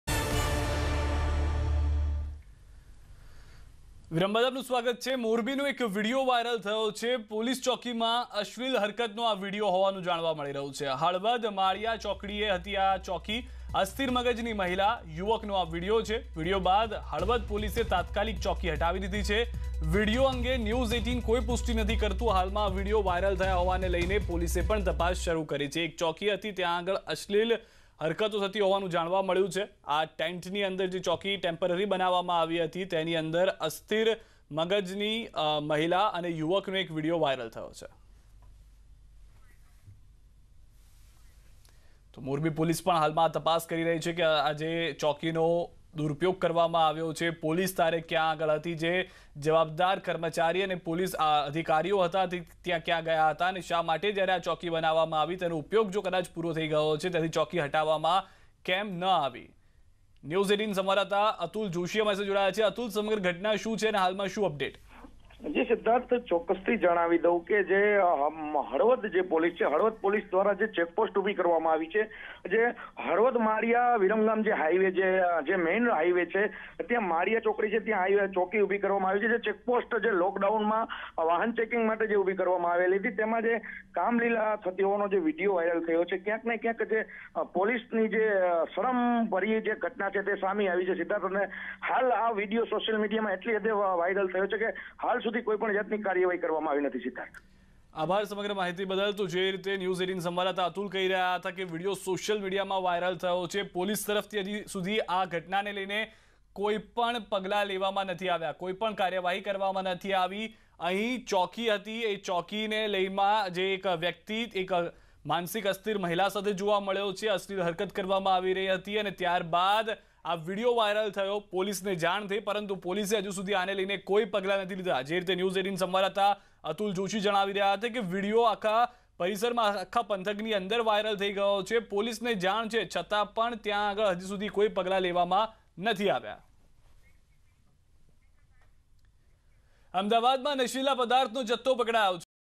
મોરબીની હળવદ ની ચેકપોસ્ટનો અશ્લીલ વિડીયો વાયરલ :
મોરબીની હળવદ પોલીસ સંચાલિત મોરબી ચોકડી પરની ચેક પોસ્ટમાં અશ્લીલ હરકતોનો વિડીયો વાયરલ થતાં ચકચાર